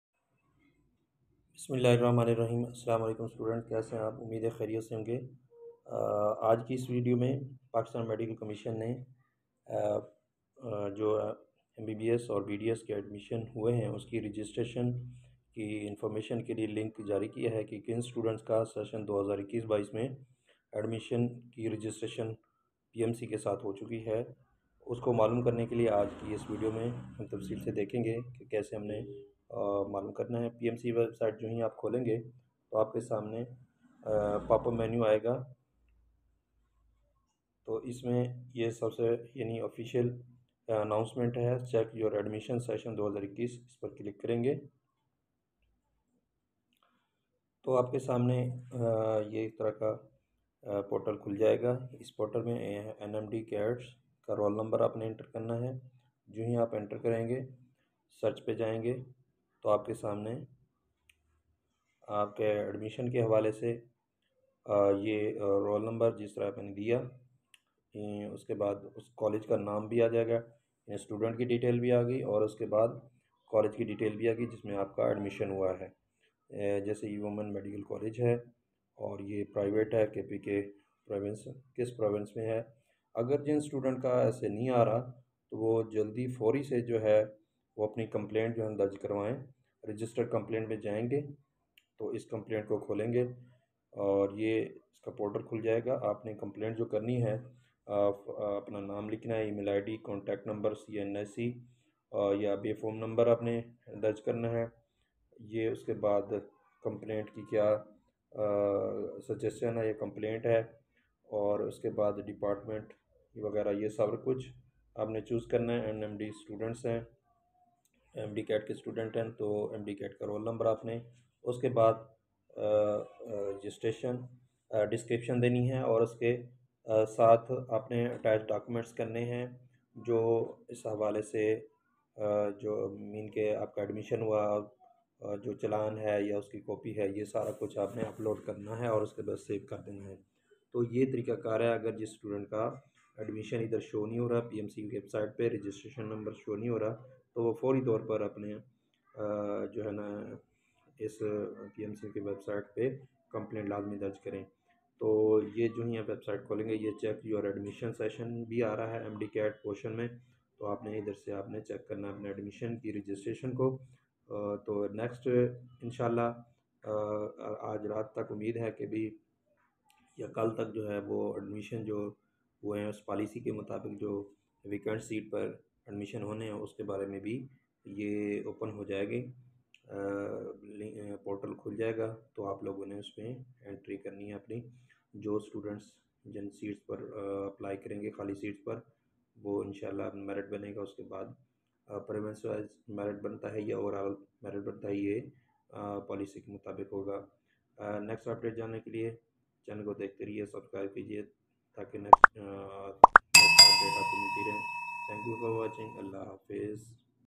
How to check and Verify Your PMC Admission MBBS BDS 2021-22 in Medical Colleges| How Apply Complaint
PUBLIC ANNOUNCEMENT

How to check PMC admission registration MBBS BDS 2021-22